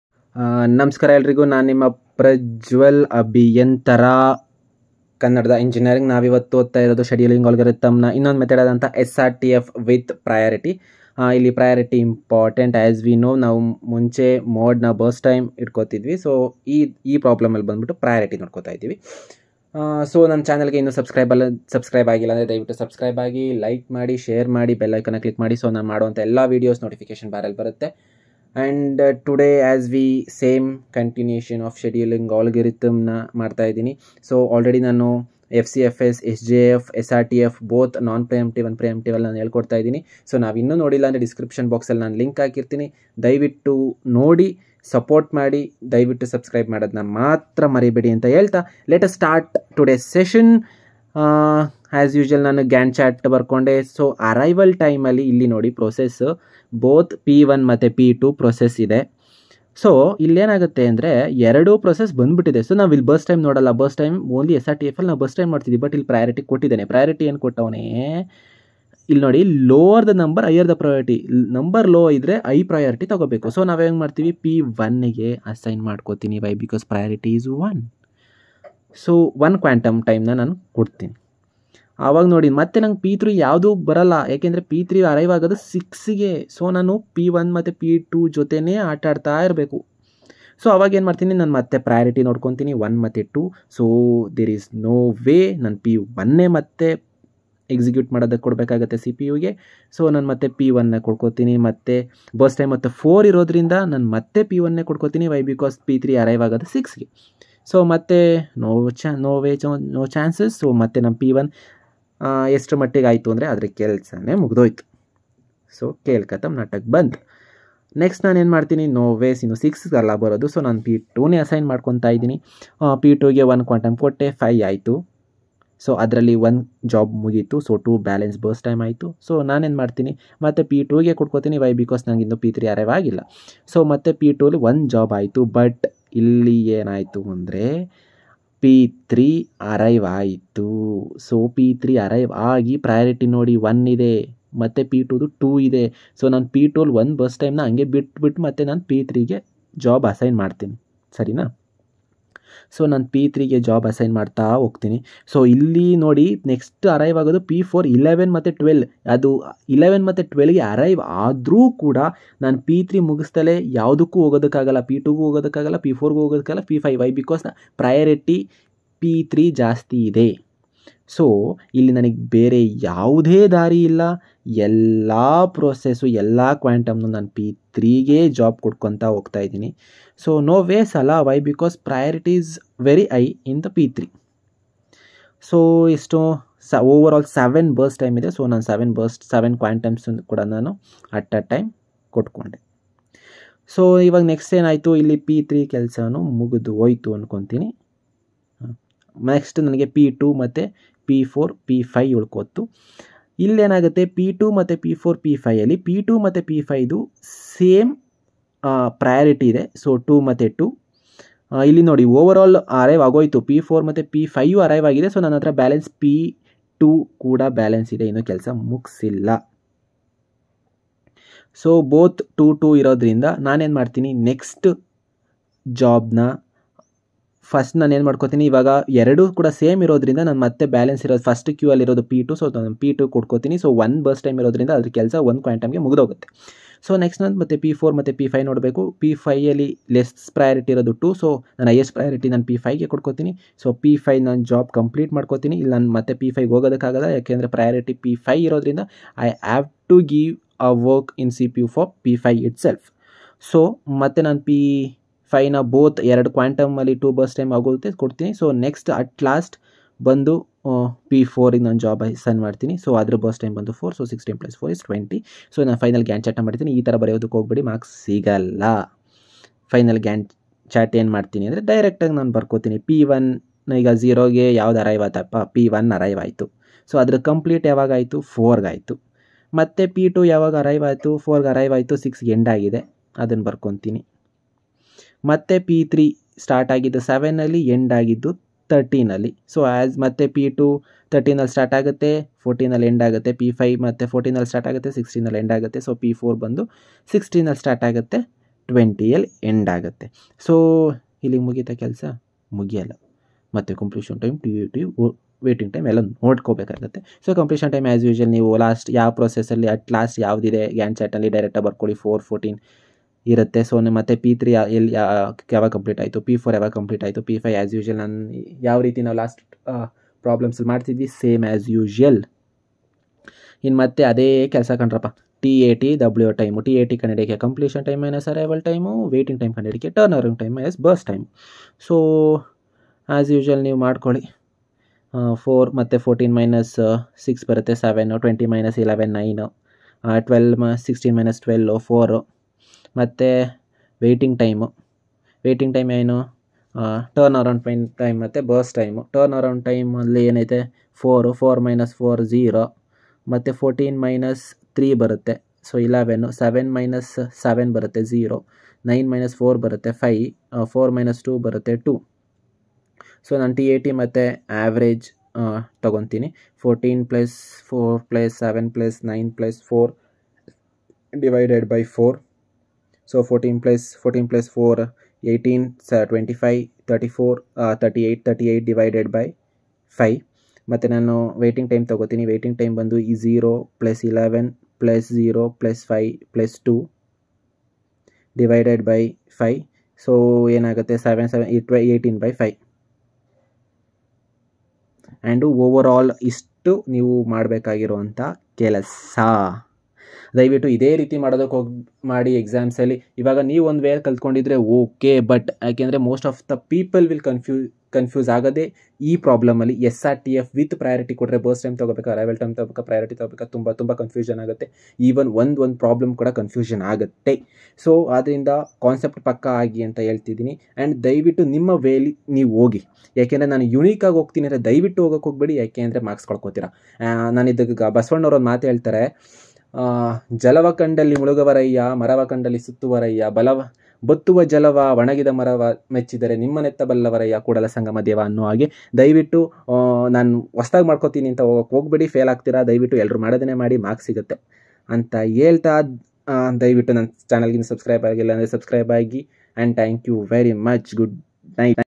SRTF with Priority(ಕನ್ನಡದಲ್ಲಿ)Shortest Remaining Time First in Scheduling algorithm,Operating System
Shortest remaining time, also known as shortest remaining time first (SRTF), is a scheduling method that is a preemptive version of shortest job next scheduling. In this scheduling algorithm, the process with the smallest amount of time remaining until completion is selected to execute.